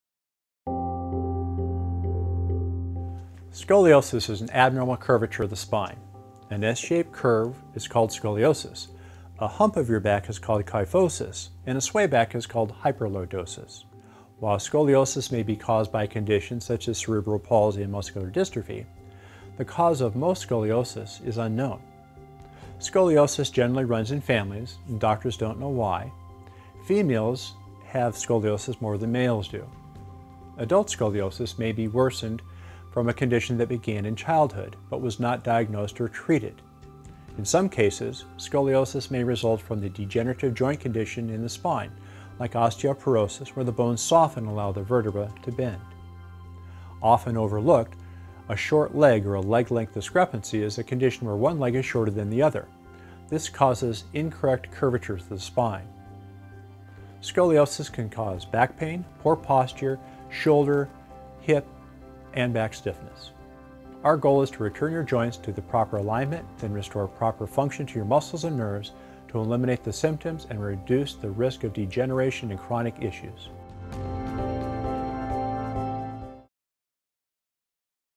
Scoliosis Treatment | Dr. Winchell
Scoliosis is an abnormal curve of the spine. Symptoms of scoliosis include pain and changes with walking. Signs include one hip or shoulder looking higher, head not looking centered, length of one pant looking shorter. Book Online for Free Consult with a Scoliosis Chiropractor in Orange County